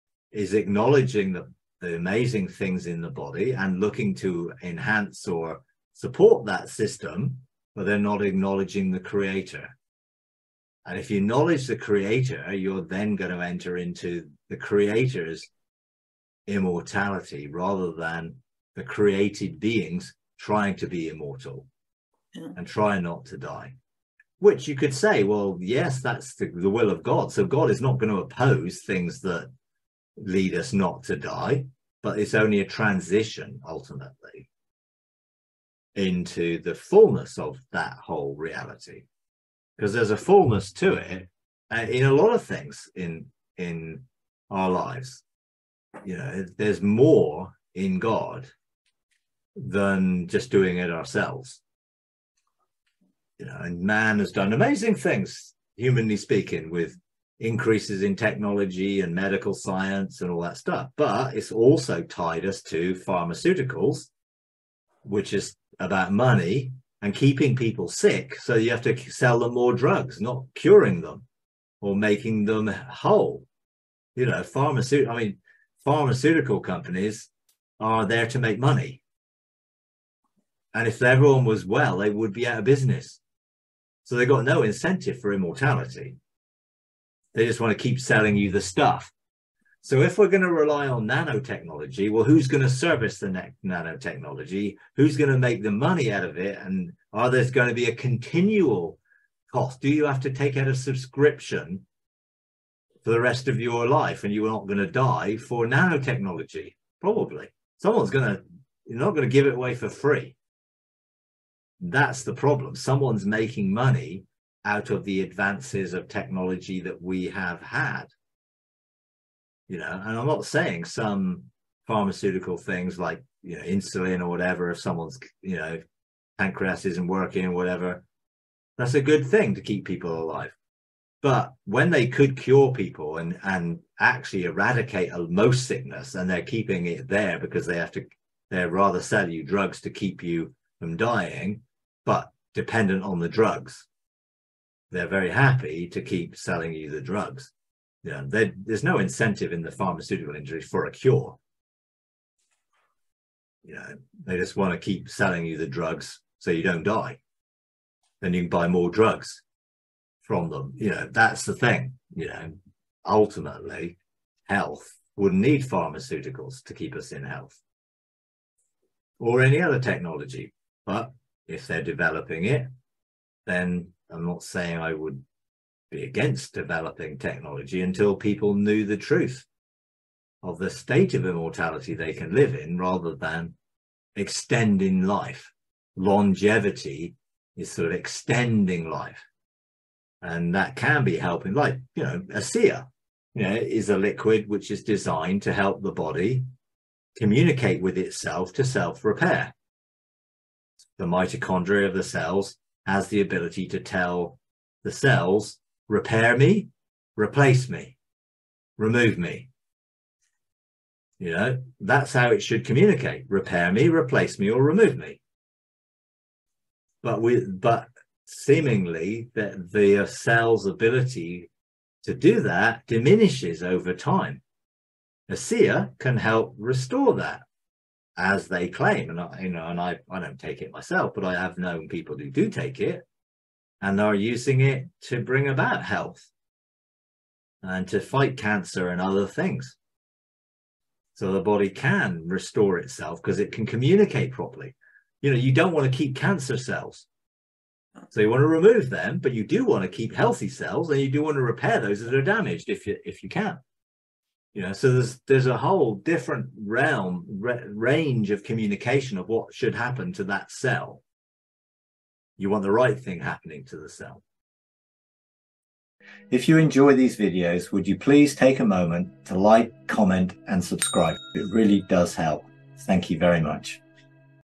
Rethinking Immortality: The Impact of Technology on Humanity
Mike explores the intersection of technology, immortality, and the role of pharmaceutical companies in modern society and delves into the importance of acknowledging the Creator in technological advancements and questions the motives behind the pharmaceutical industry's focus on profit over healing.

1. Introduction to Technological Advancements - 00:00
2. Acknowledging the Creator in Innovation - 00:45
3. Immortality and the Role of Technology - 01:30
4. Critique of Pharmaceutical Industry - 02:15
5. Rethinking Nanotechnology - 03:15
6. Conclusion and Call to Action - 04:00